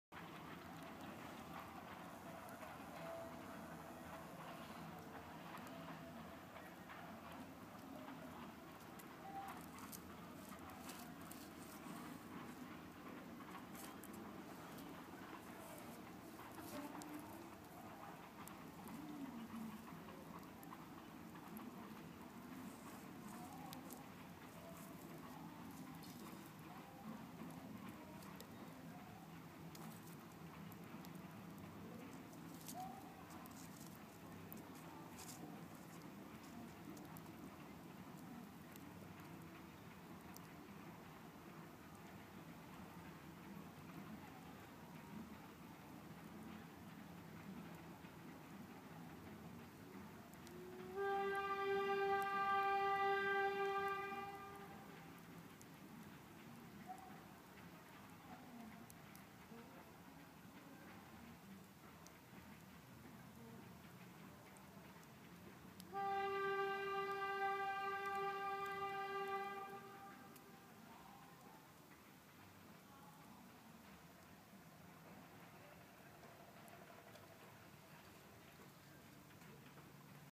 Friday Night At The Faithfulls
Sitting around our campfire and next you here is an Ol train whistle, the clackety clack and music :) it's the Blue Train! Very cool!